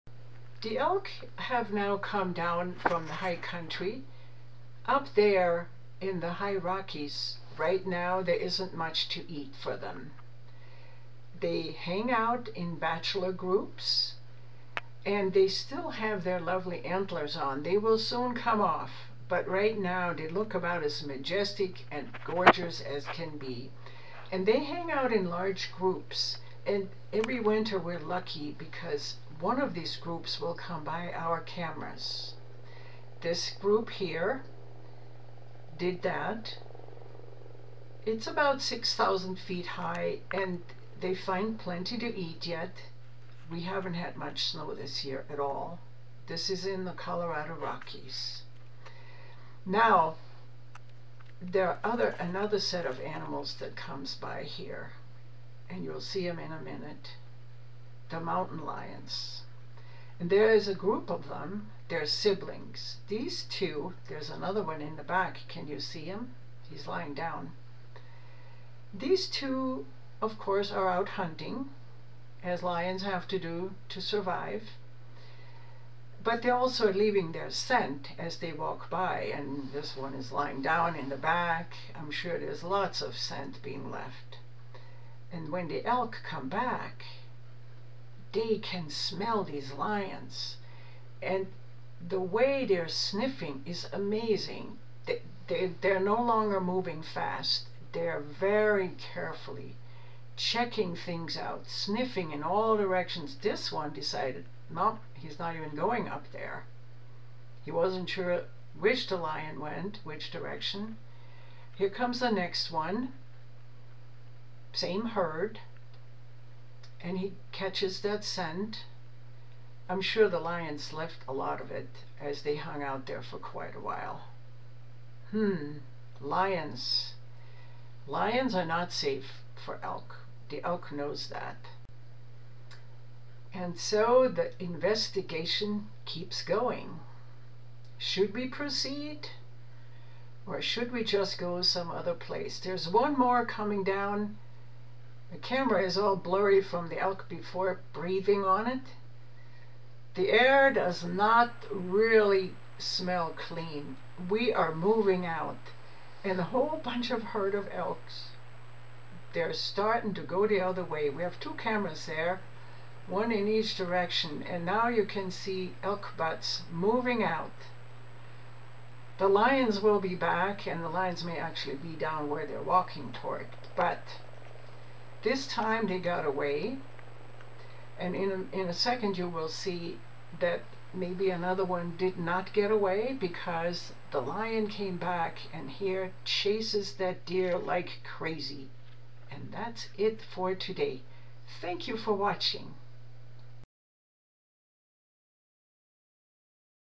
Sound up. A large herd of bull elk comes upon an area marked by mountain lions
In the winter the bull elk hang out in bachelor groups and come down lower into the Foothills. This group happened upon a trail, where two mountain lions had been hunting. Watch to the end!